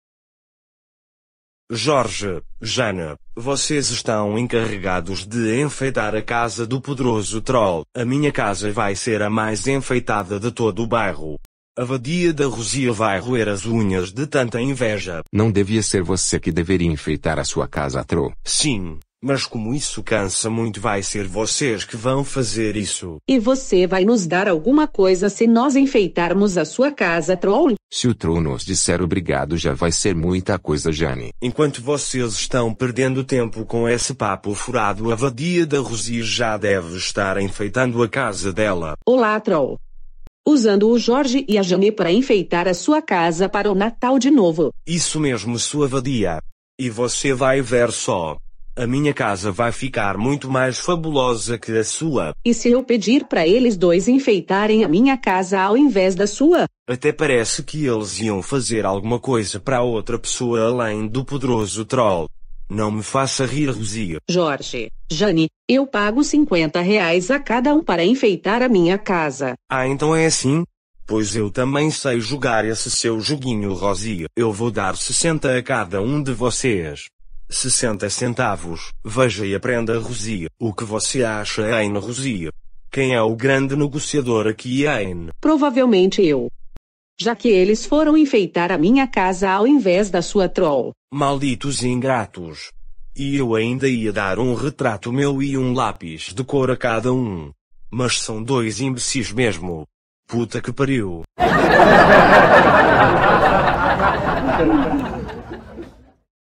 Enfeitando Casas
Qual casa que o Jorge e a Jane vão colocar os enfeites natalinos?